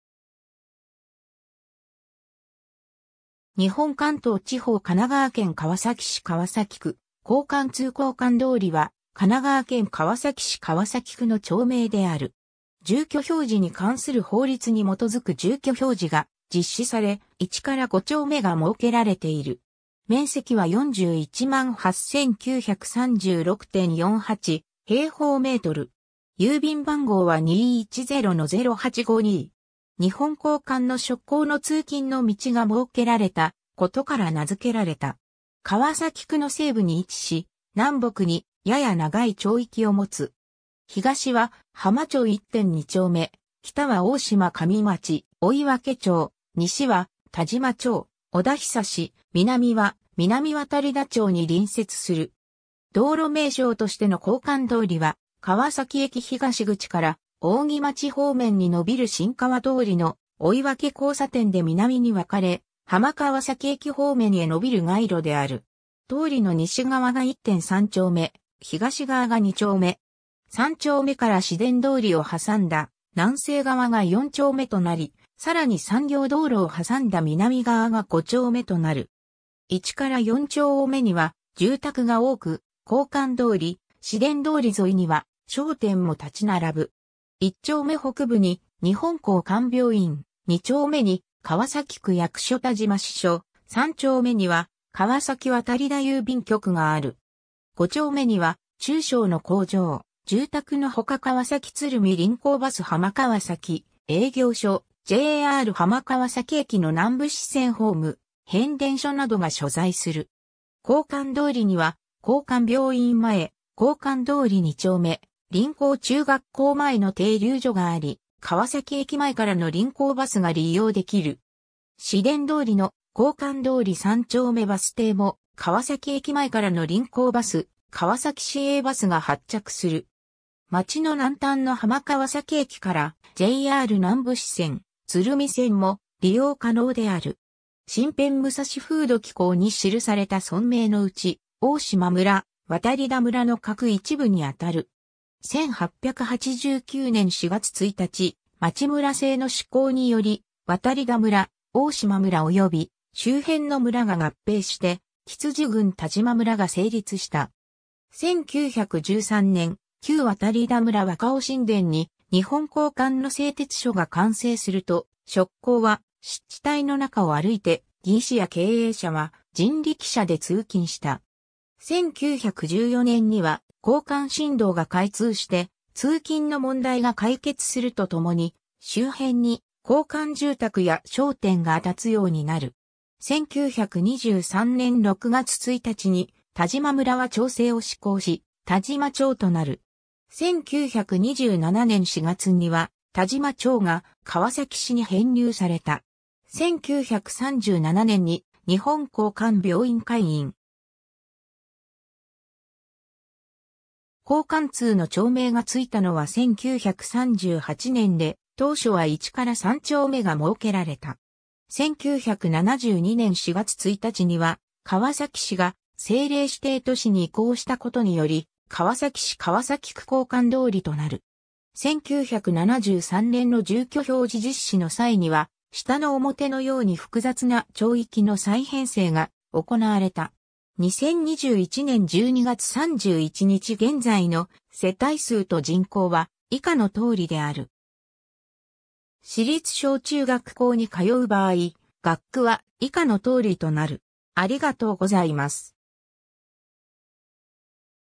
鋼管通
川崎区の町・字
日本 」 関東地方 」 神奈川県 」 川崎市 」 川崎区 」 鋼管通 鋼管通（こうかんどおり）は、神奈川県川崎市川崎区の町名である。
住居表示に関する法律に基づく住居表示が実施され、1～5丁目が設けられている。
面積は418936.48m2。
日本鋼管の職工の通勤の道が設けられたことから名づけられた。
 川崎区の西部に位置し、南北にやや長い町域を持つ。
東は浜町1・2丁目、北は大島上町・追分町、西は田島町・小田栄、南は南渡田町に隣接する。
 道路名称としての鋼管通りは、川崎駅東口から扇町方面に伸びる新川通りの追分交差点で南に分かれ、浜川崎駅方面へ伸びる街路である。
通りの西側が1・3丁目、東側が2丁目。
3丁目から市電通りを挟んだ南西側が4丁目となり、さらに産業道路を挟んだ南側が5丁目となる。
1～4丁目には住宅が多く、鋼管通り・市電通り沿いには商店も立ち並ぶ。
1丁目北部に日本鋼管病院、2丁目に川崎区役所田島支所、3丁目には川崎渡田郵便局がある。
5丁目には中小の工場・住宅のほか川崎鶴見臨港バス浜川崎営業所、JR浜川崎駅の南武支線ホーム、変電所などが所在する。
 鋼管通りには「鋼管病院前」「鋼管通2丁目」「臨港中学校前」の停留所があり、川崎駅前からの臨港バスが利用できる。
市電通りの「鋼管通3丁目」バス停も、川崎駅前からの臨港バス・川崎市営バスが発着する。
町の南端の浜川崎駅からJR南武支線・鶴見線も利用可能である。
 『新編武蔵風土記稿』に記された村名のうち、大島村・渡田村の各一部にあたる。
1889年（明治22年）4月1日、町村制の施行により渡田村、大島村および周辺の村が合併して橘樹郡田島村が成立した。
1913年（大正2年）、旧渡田村若尾新田に日本鋼管の製鉄所が完成すると、職工は湿地帯の中を歩いて、技師や経営者は人力車で通勤した。
1914年には鋼管新道が開通して通勤の問題が解決するとともに、周辺に鋼管住宅や商店が建つようになる。
 1923年（大正12年）6月1日に田島村は町制を施行し、田島町となる。
1927年（昭和2年）4月には、田島町が川崎市に編入された。
1937年に日本鋼管病院開院。
鋼管通の町名がついたのは1938年（昭和13年）で、当初は1～3丁目が設けられた。
1972年（昭和47年）4月1日には川崎市が政令指定都市に移行したことにより、川崎市川崎区鋼管通となる。
1973年の住居表示実施の際には下の表のように複雑な町域の再編成が行われた。
 2021年（令和2年）12月31日現在の世帯数と人口は以下の通りである 。